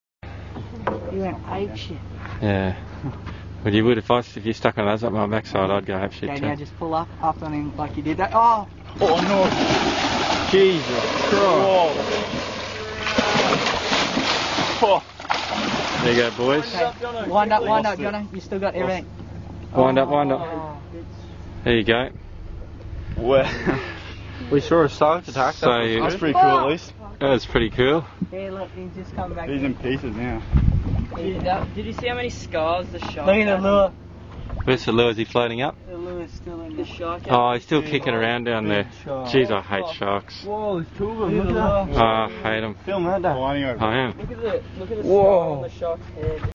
Shark Steals Caught Fish
A hungry shark jumps out of a nowhere and snatches a mackerel fish straight from the line!